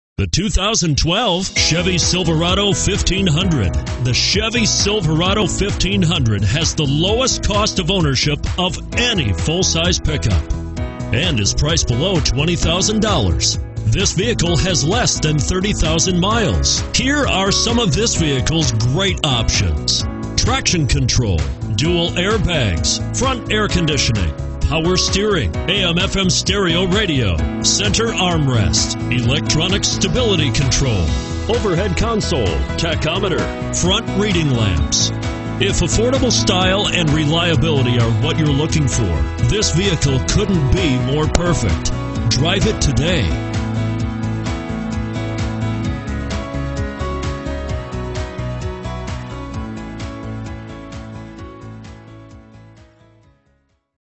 2012 Chevrolet Silverado 1500 Scottsboro AL 10T016I
2012 Chevrolet Silverado 1500 Work Truck - $18532

4-Speed Automatic with Overdrive. Surround soundlessness. A true master of tasks. brbrWork-a-day drivers find a new best friend in the Silverado 1500. Named a 2010 Consumer Guide recommended Buy. It is nicely equipped with features such as 4-Speed Automatic with Overdrive. With plenty of passenger room, you won't have to worry about being cramped when it's more than just you in the truck. brbrDELIVERY IS AVAILABLE. JUST LET US KNOW HOW WE CAN BETTER SERVE YOU.